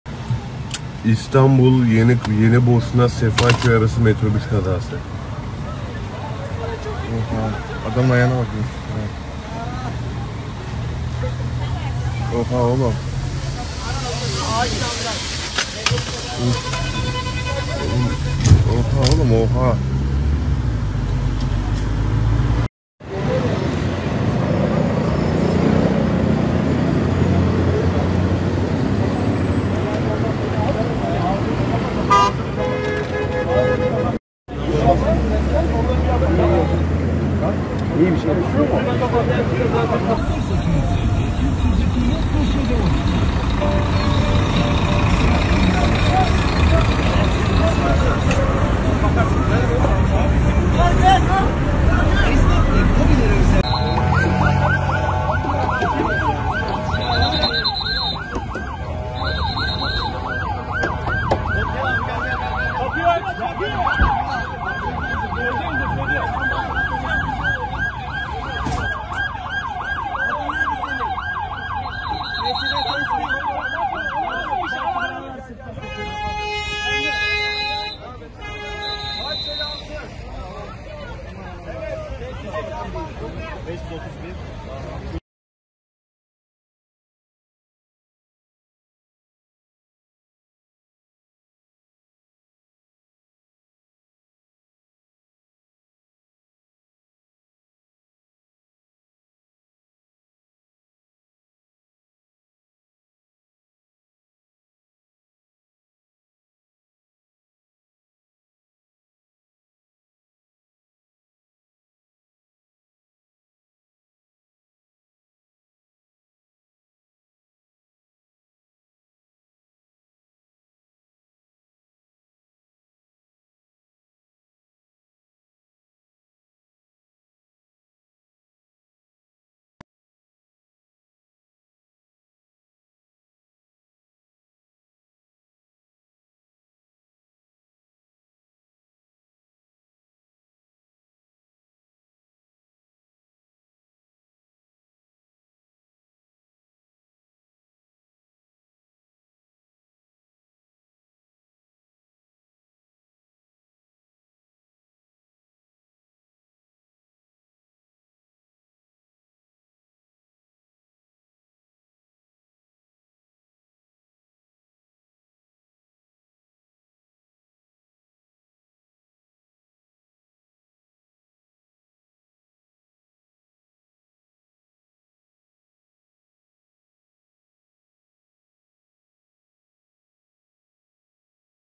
İstanbul'daki Korkunç Metrobüs Kazasında Yaralılar Var! Facianın Eşiğinden Dönüldü!
Küçükçekmece''de metrobüs kaza yaptı. Yaralılar var. Olay yerine itfaiye, sağlık ve polis ekipleri sevk edildi. Yaralılar olay yerine gelen sağlık ekipleri tarafından ambulansla çevredeki hastanelere kaldırıldı.